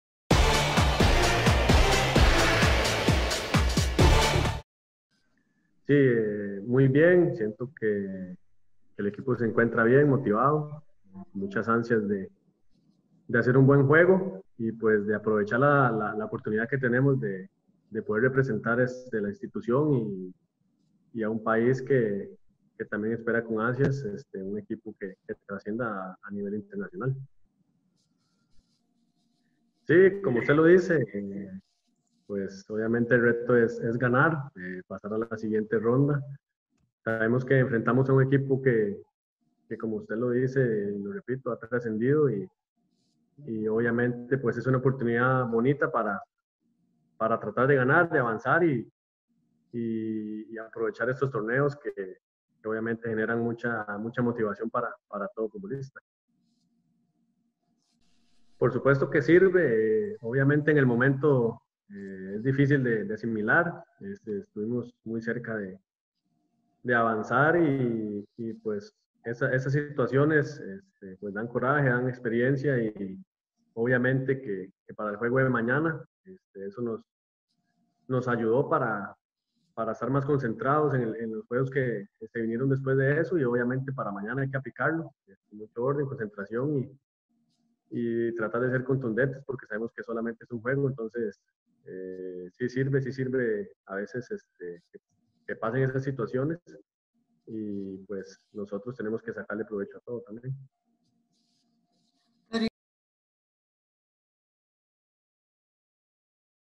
JOSÉ MENA: "EL RETO ES GANAR Y AVANZAR"
José Antonio Mena Alfaro considera que el juego ante el club panameño CAI es una buena oportunidad para que Antigua GFC pueda trascender en el fútbol de la región.